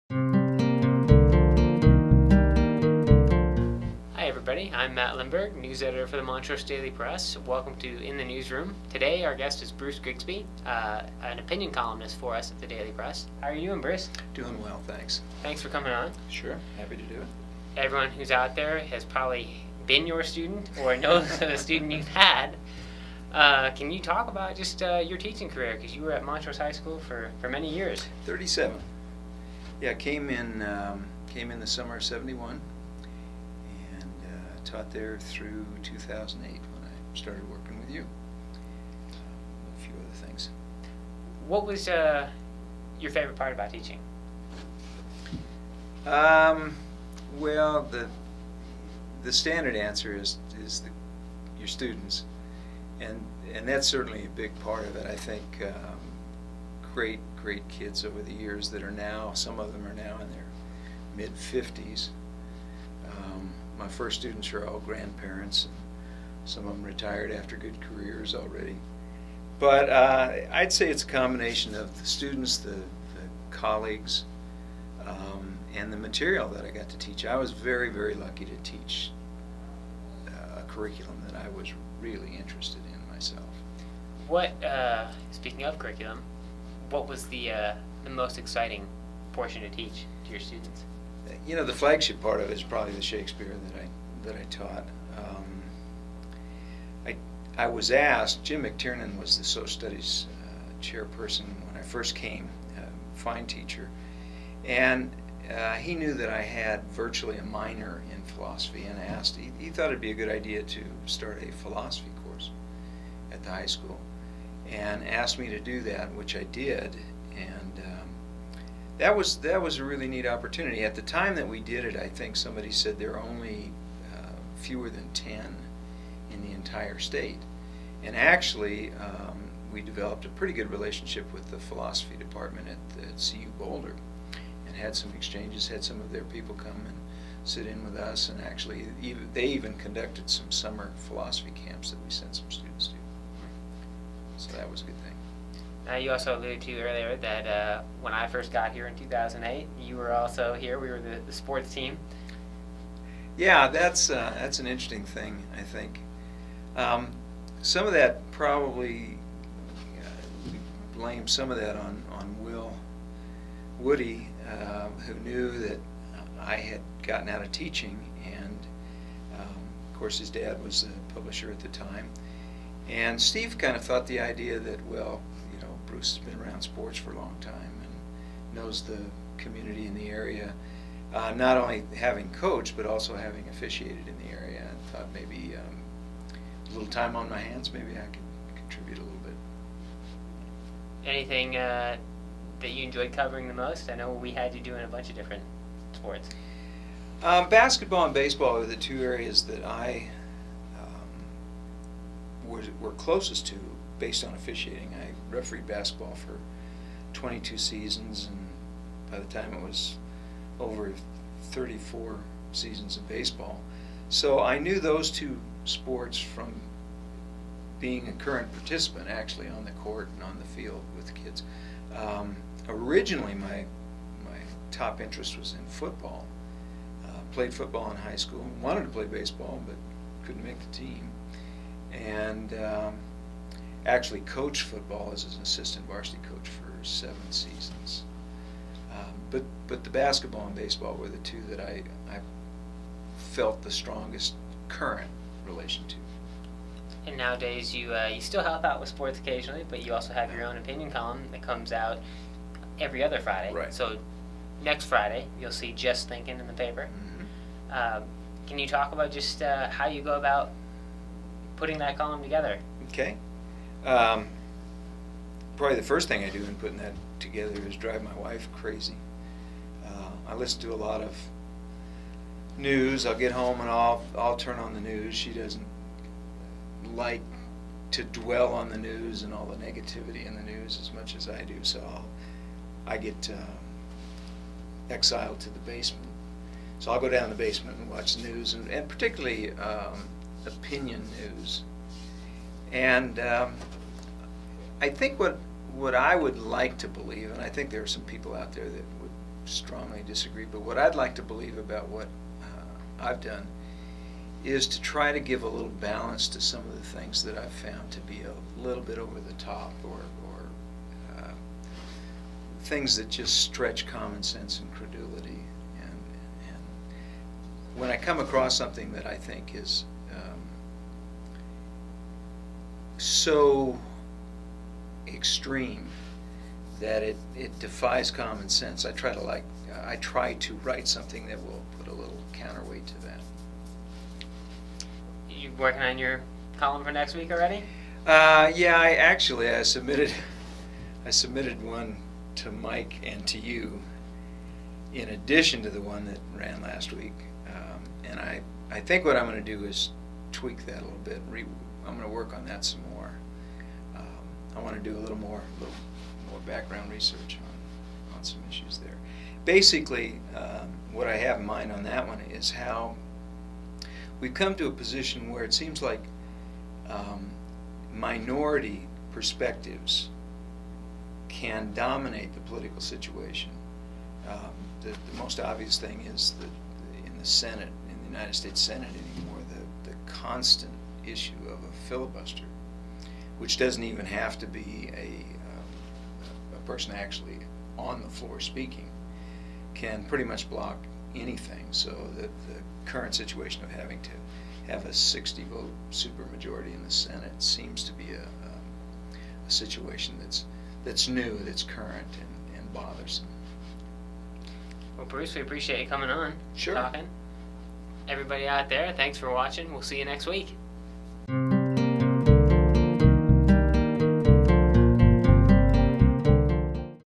4-26 In the Newsroom
Matt talks to retired teacher and current columnist, Bruce Grigsby about sports, writing and his teaching career.